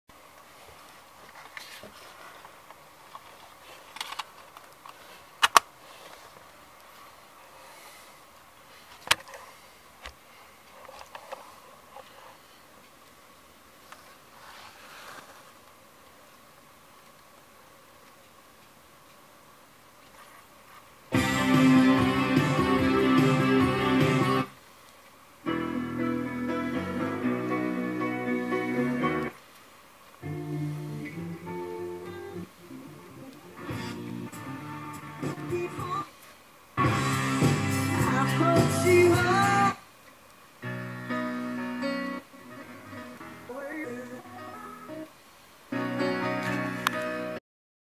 DEMO OF SONY MD WALKMAN SONY MZ-R30 MINIDISC RECORDER FOR SALE
DEMO OF A SONY MD WALKMAN SONY MZ-R30 MINIDISC RECORDER FOR SALE by SOHOAUCTION on EBAY